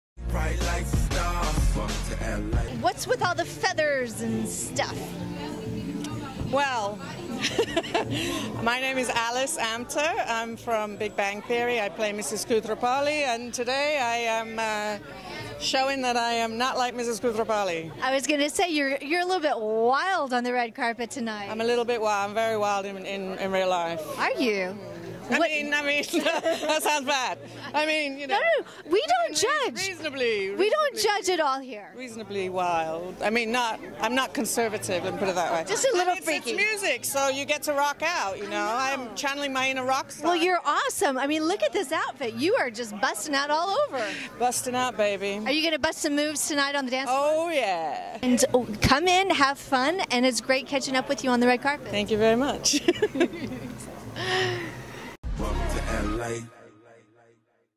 ISL Live from the BET True Magazine with Alice Amter
Kelly Koppell interviewing Actress Alice Amter from The Big Bang Theory, live from the red carpet during BET Awards week at the True Magazine Party.